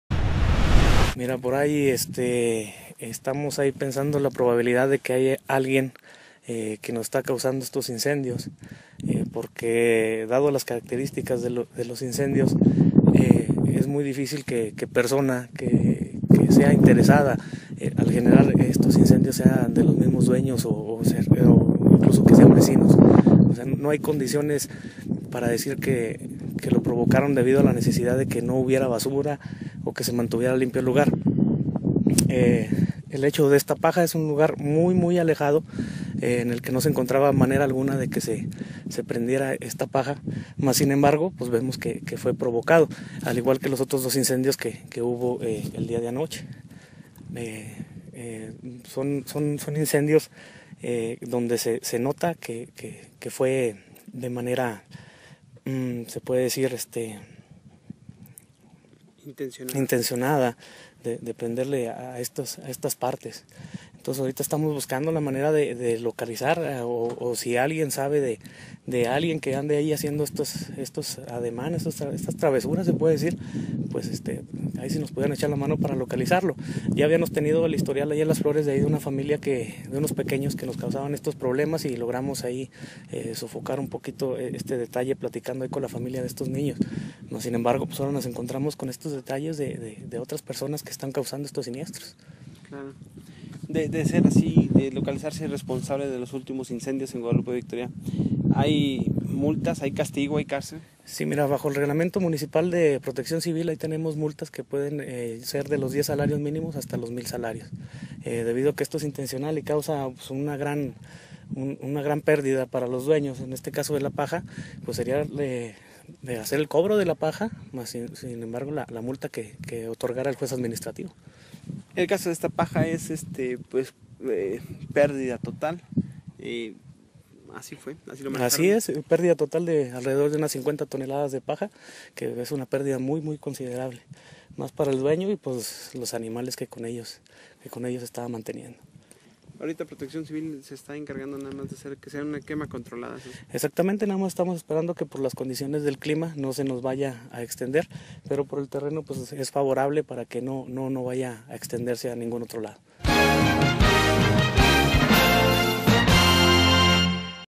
GEHIEL QUIÑONES
Guadalupe Victoria, Dgo.- En menos de 24 horas se registraron tres incendios en las afueras de la cabecera municipal, el último es una paja de alrededor de 50 toneladas que a decir de Jehiel Quiñones, titular de la Unidad de Protección Civil (UPC), no había razón alguna para que esto sucediera, a menos que haya sido intencionado, de ahí que se sospecha de una o varias personas que han ocasionado los últimos incendios